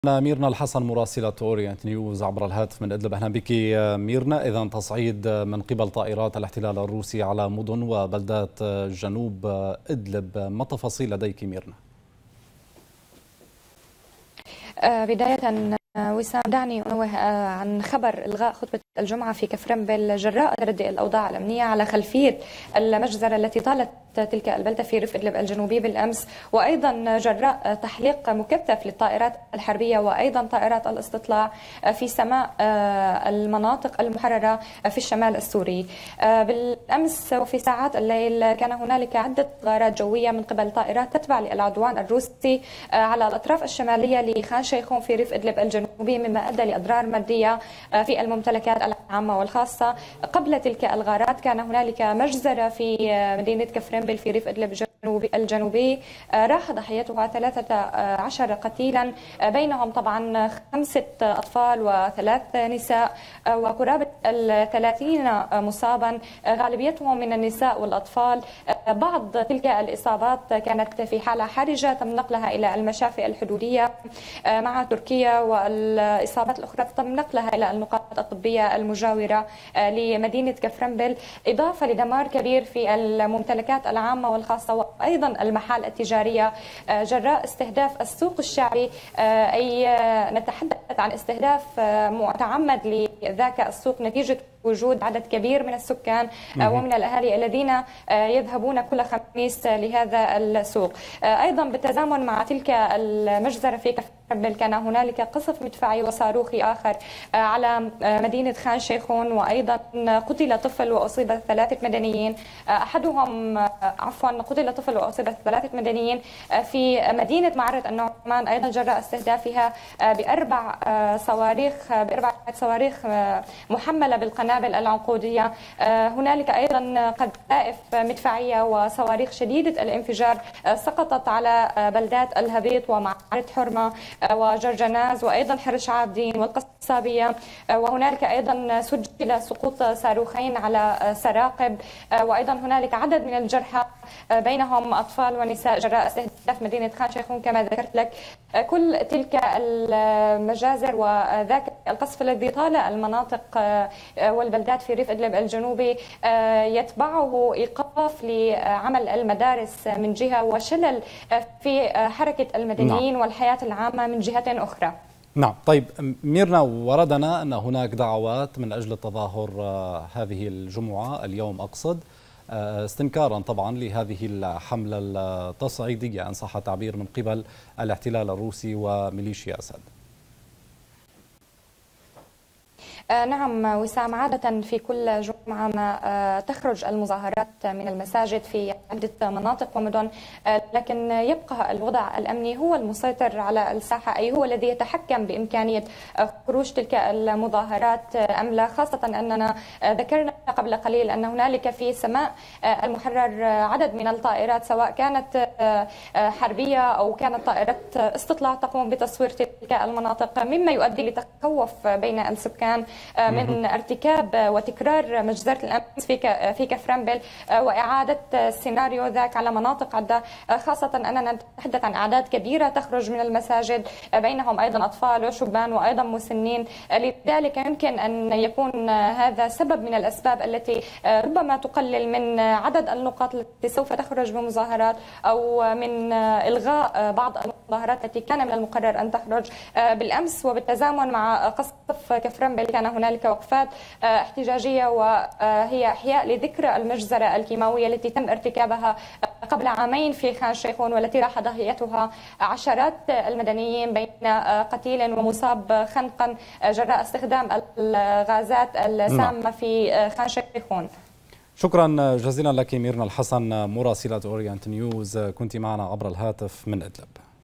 الاحتلال الروسي يكثف غاراته الجوية على مدن وبلدات جنوب إدلب
أفادَ مراسلُ أورينت نيوز بشنِ الاحتلاِل الروسي أربعَ غاراتٍ جوية على الأطرافِ الشمالية من مدينةِ خان شيْخُون جنوبَ إدلب ما تسببَ بأضرارٍ مادية فقط. إلى ذلك ارتفعت حصيلةُ قصفِ ميليشيا أسد على السوقِ الشعبي في مدينةِ كَفرنْبِل إلى ثلاثةَ عشرَ قتيلاً مدنياً إضافةً لعشراتِ الجرحى.. القصفُ طالَ أيضاً مدينتي معرة النعُمان وسراقِب وبلدةَ جرجَناز وقريةَ معرة حرمة وحرش القصابية و بلدةَ الهبيط بقذائفَ شديدةِ الانفجار وقذائفَ تحملُ قنابلَ عنقودية.
ميرنا الحسن - مراسلة أورينت نيوز - إدلب -dtl